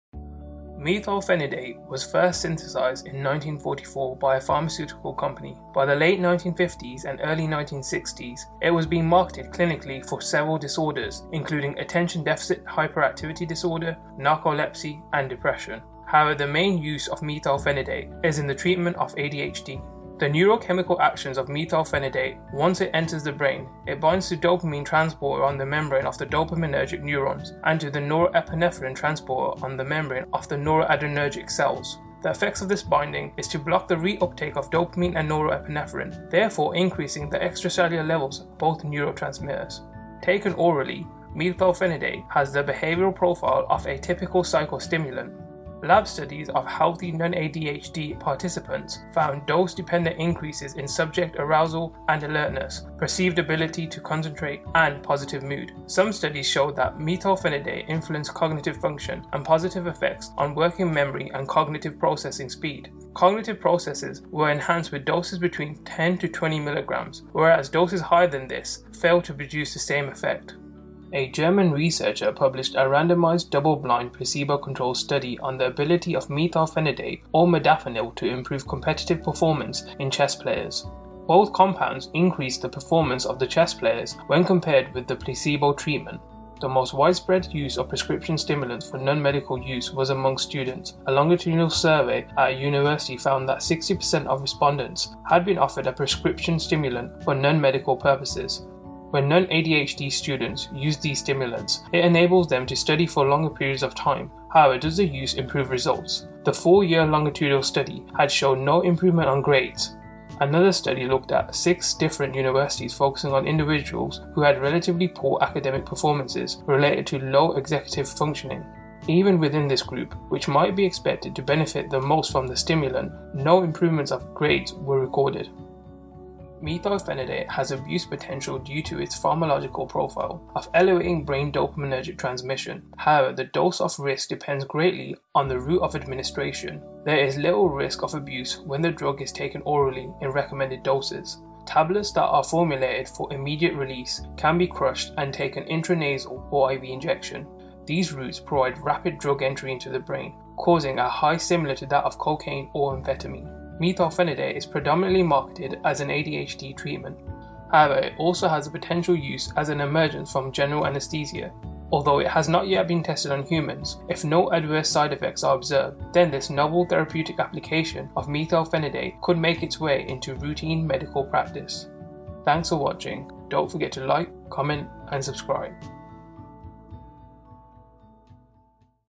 Methylphenidate
Hello and welcome to Bear Explanations! In this video we explore the effects of methylphenidate on the brain, it uses and the potential use in the future.